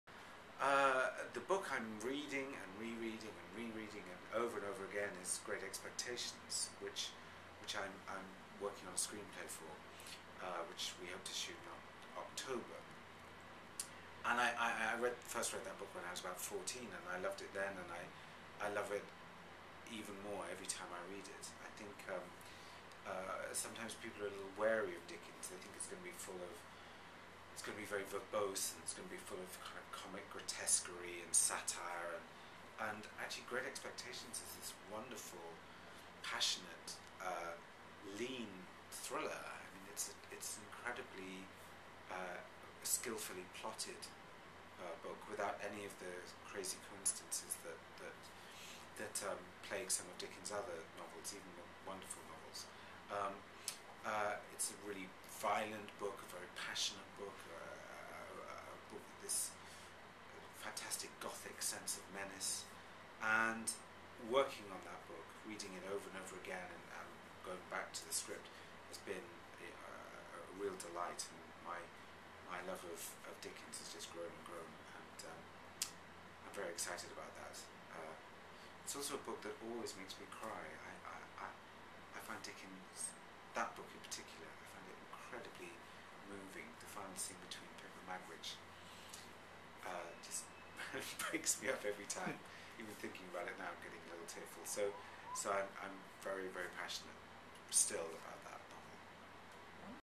Whatcha Reading, David Nicholls?
Novelist and screenwriter David Nicholls (ONE DAY) tells us all about his enduring love for Charles Dickens' GREAT EXPECTATIONS -- which he's currently adapting for a 2012 movie starring Helena Bonham Carter and Ralph Fiennes.